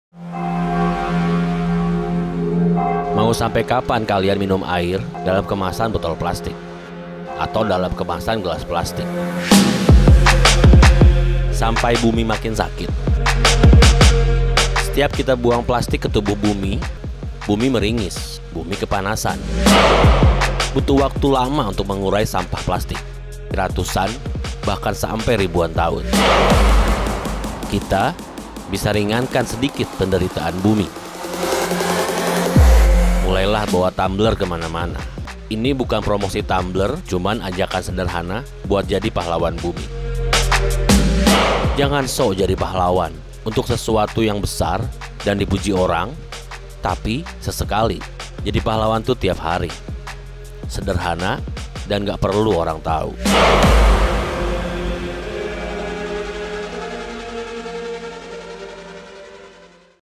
STOP BELI MINUMAN DALAM KEMASAN PLASTIK!! Yuk Selamatkan Bumi
Menteri Kelautan dan Perikanan Susi Pudjiastuti mengimbau agar masyarakat tak lagi membeli minuman mineral dalam botol plastik, dan menggantinya dengan tumblr. Ini dilakukan karena banyaknya sampah plastik seperti minuman botol ke laut.

Tutup botol plastik yang ukurannya kecil tak sengaja dimakan olah penghuni laut termasuk burung dan ikan. Wajar saja, jika tutup botol ini berada di posisi kelima dari daftar plastik berbahaya di lautan.